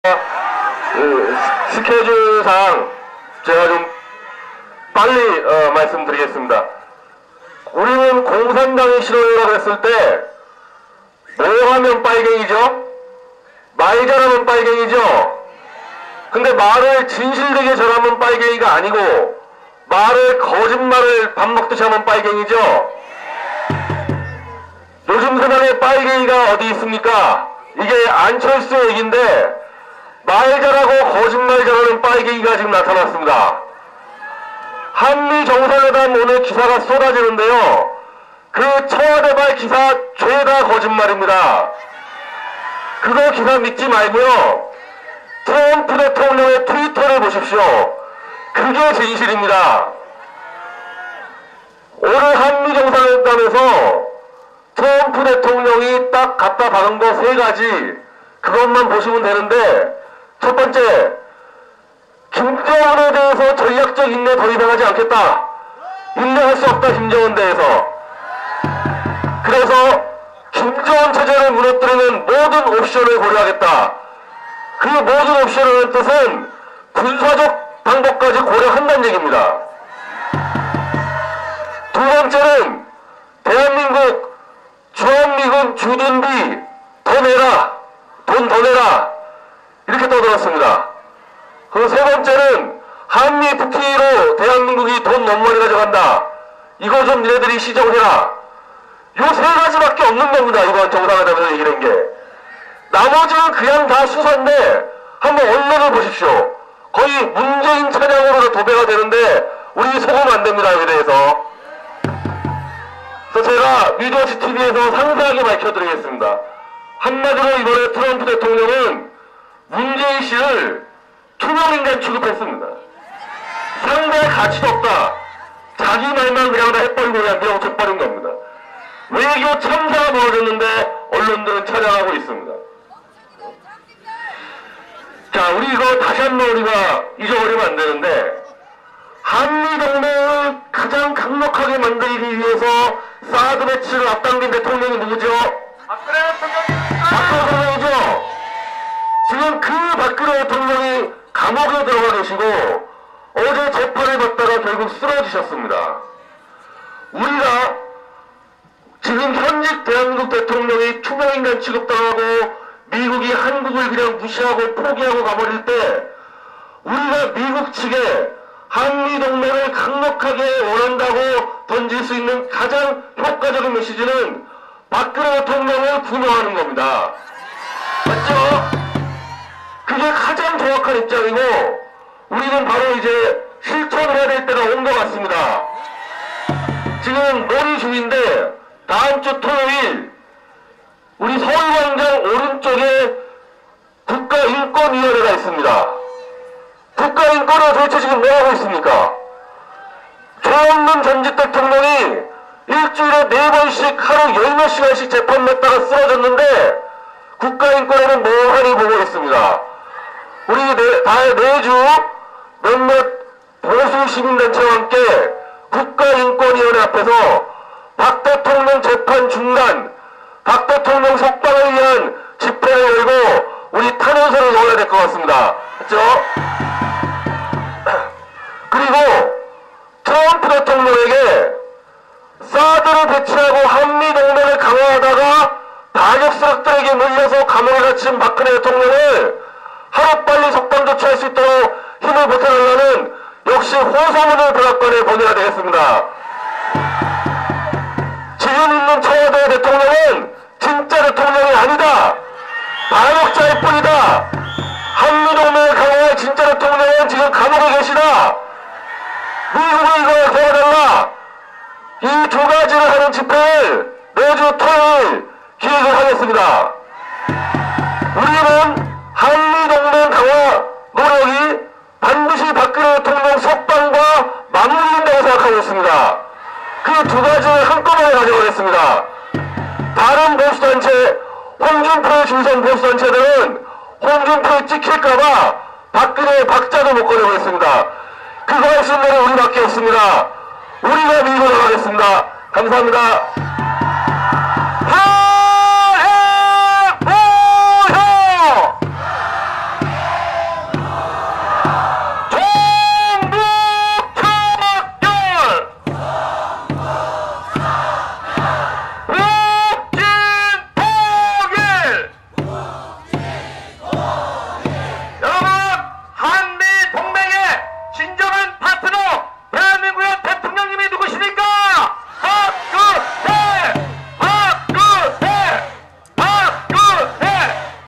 평창_ 미국에서 투명인간 취급은 누구? _ 변희재
7월 첫째주 토요일인 1일 오전 강원도 평창군 소재 이승복기념관 주차장의 집회에서 문재인의 방미에 대해 변희재는 말한다.